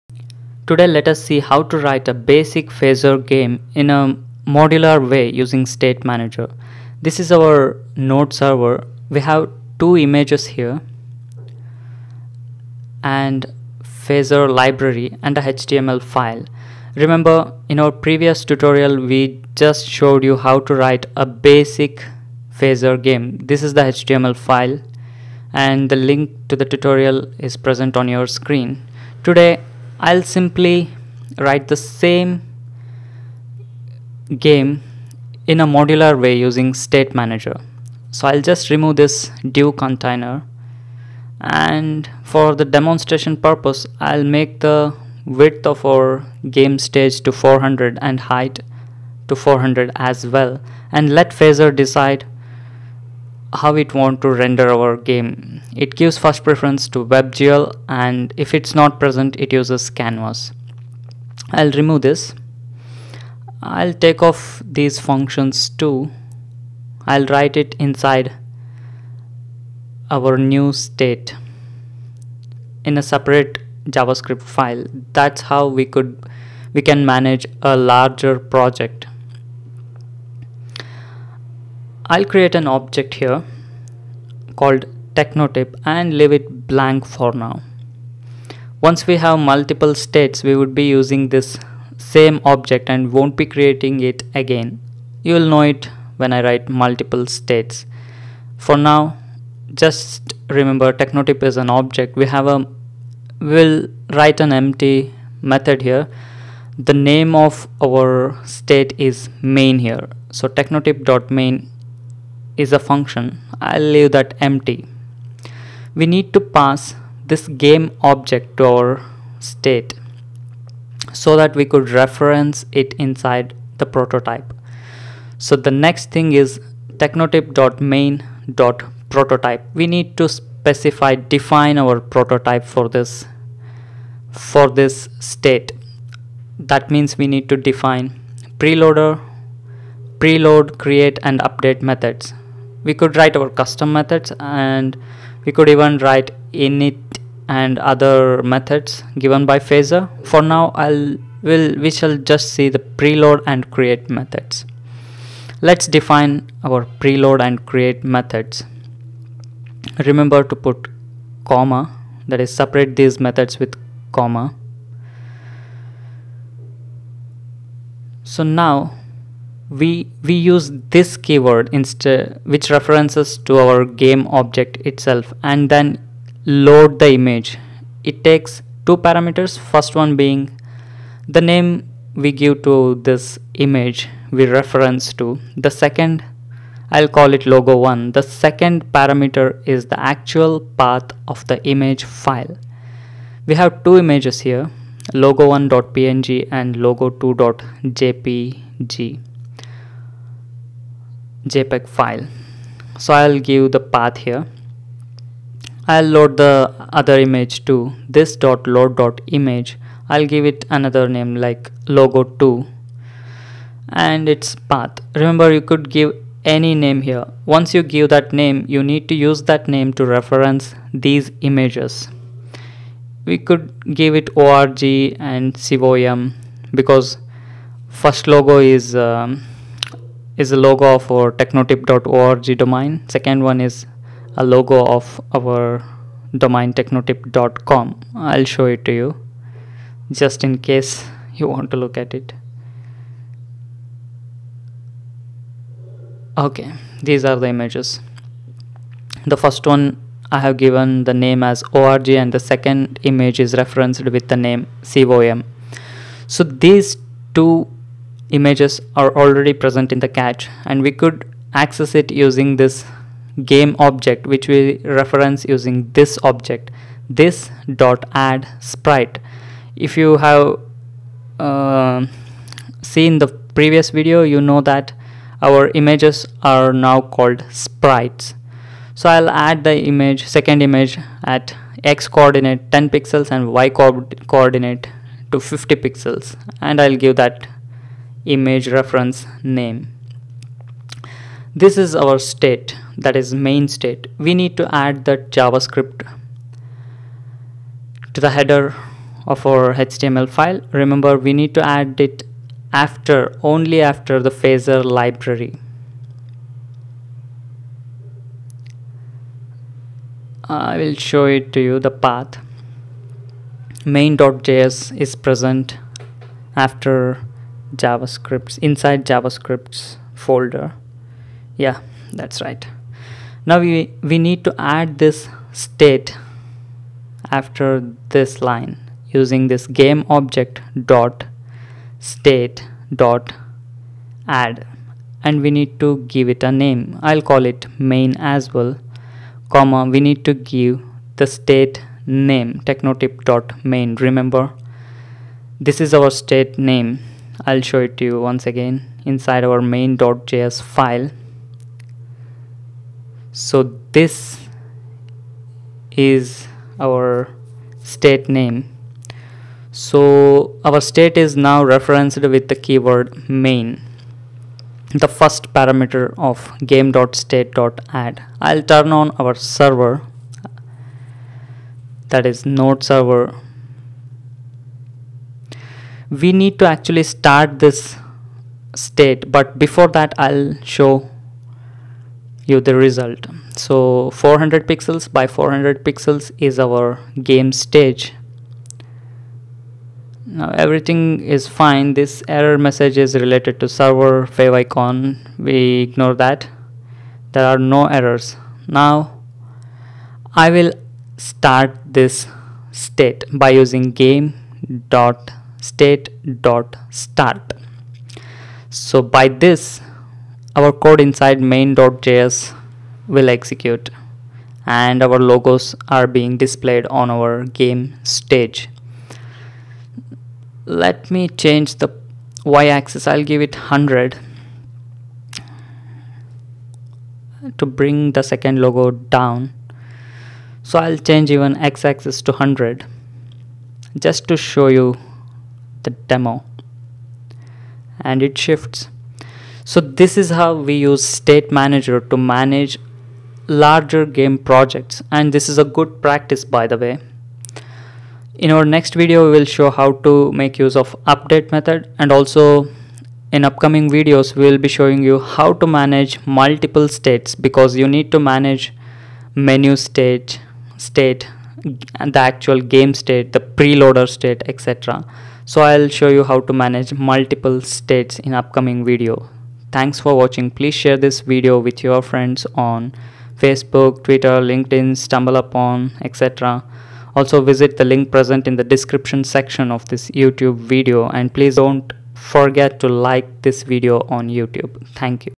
Phaser State Manager: Basic
When we have to develop a large project we need to go with modular programming – we need to break the game into several meaningful modules and develop it separately and then let them communicate to produce the required results.